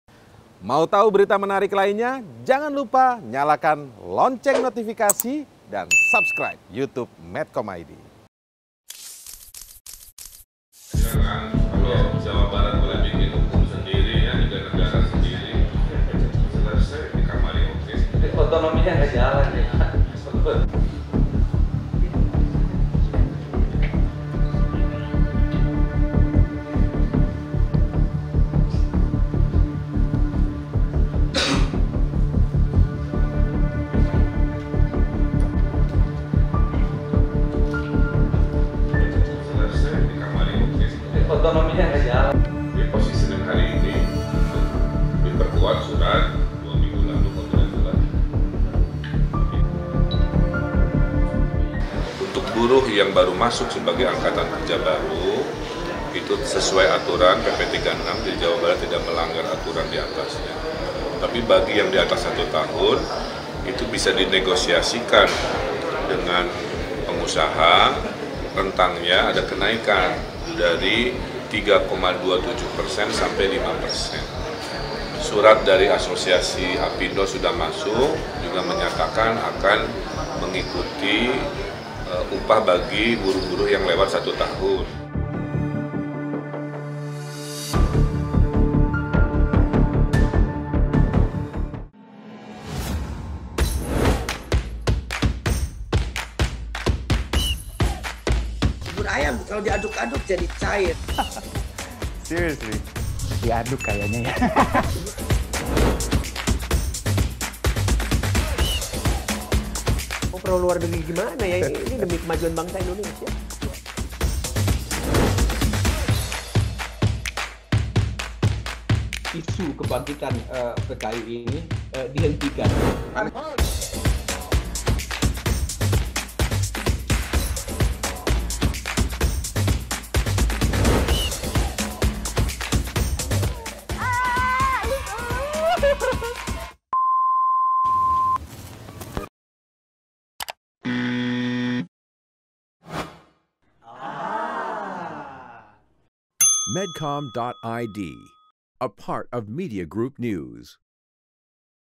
Langkah Anies Baswedan Naikkan Upah Buruh 5% Diikuti Ridwan Kamil
Keputusan Gubernur DKI Jakarta Anies Baswedan menaikkan upah buruh sebanyak 5 persen diikuti oleh Gubernur Jawa Barat Ridwan Kamil. Dalam keputusannya, RK menaikkan upah 3,27-5 persen khusus bagi buruh yang telah bekerja di atas 1 tahun. Sementara upah buruh yang bekerja kurang dari 1 tahun tetap mengikuti PP 36 yang dikeluarkan pemerintah pusat. Selain itu, UMK tidak naik karena mengikuti ketentuan pusat.